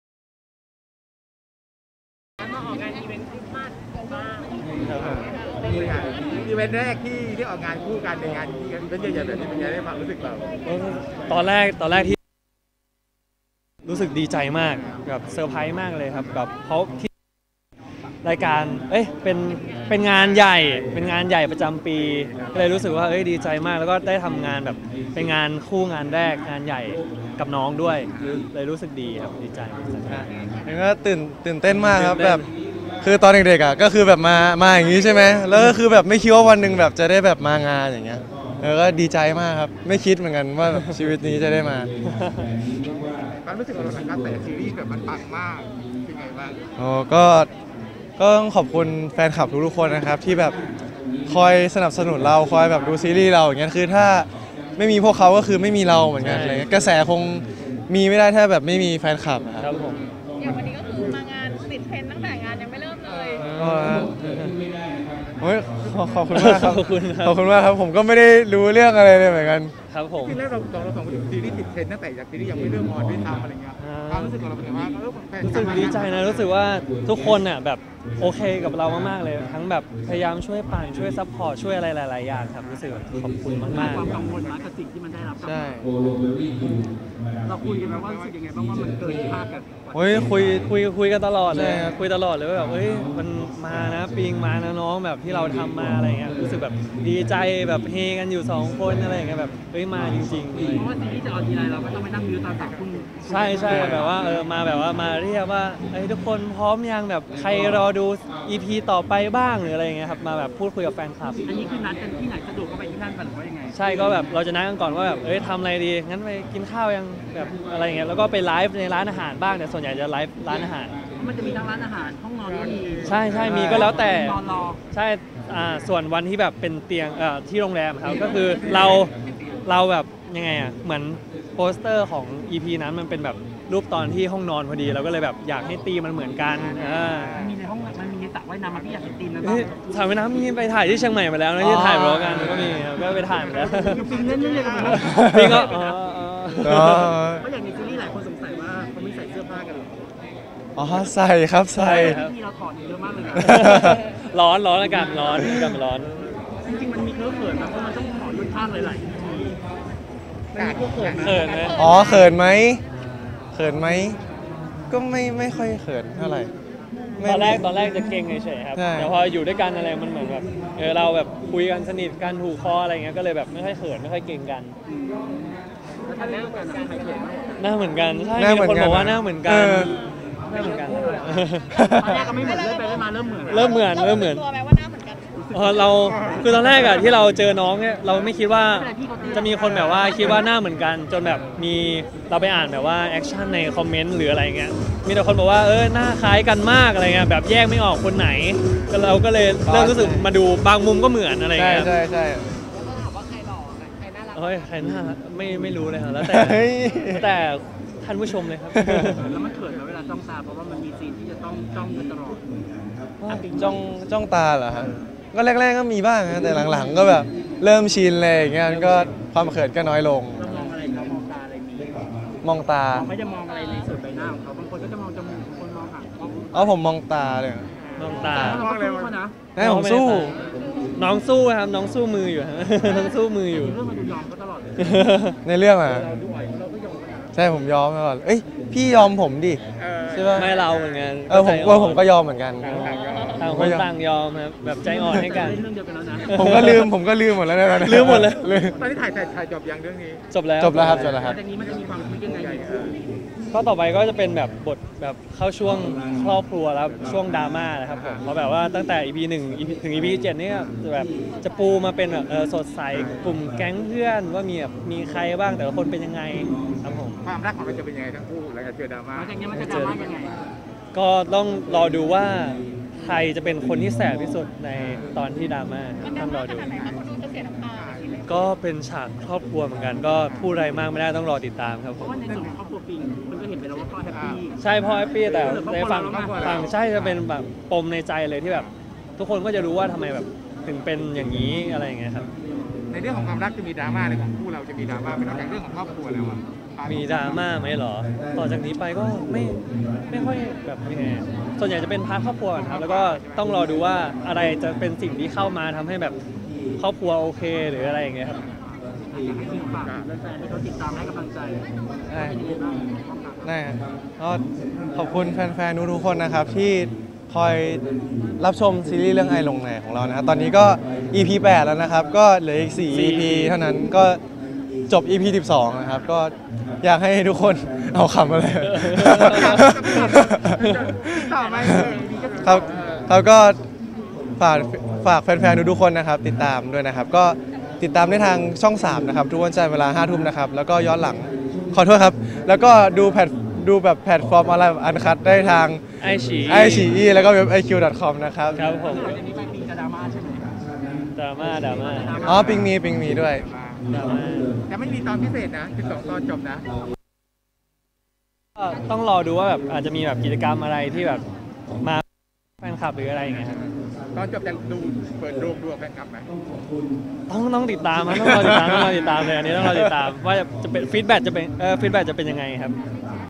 มีน นิชคุณ-ปิง กฤตนัน แฮปปี้ อัยย์หลงไน๋ กระแสดีเกิดคาด ช่วงแรกเขินฉากNC หลังๆชิน
มีน นิชคุณ ควงคู่จิ้น ปิง กฤตนัน ดีใจออกอีเว้นท์เปิดไฟต้นคริสต์มาส เผยตอนเด็กๆเคยตื่นเต้นกับเทศกาลคริสต์มาสประดับไฟที่CTW ไม่คิดว่าวันนั้นตนจะได้มีส่วนร่วม แฮปปี้อัยย์หลงไน๋ กระแสตอบรับดีเกิดที่คาดไว้ รับอ่านคอมเม้นท์จากแฟนคลับ มีนัดกันไลฟ์คุยกับแฟนๆดูซีรีส์พร้อมกันทุก3ทุ่ม เผยช่วงแรกมีเขินเข้าฉากNC หลังๆเริ่มชิน หลังจากนี้จะเริ่มมีดราม่าครอบครัวในเรื่อง ให้แฟนๆรอดูว่าจะเสียน้ำตาหนักแค่ไหน ฝากแฟนๆรอติดตามกัน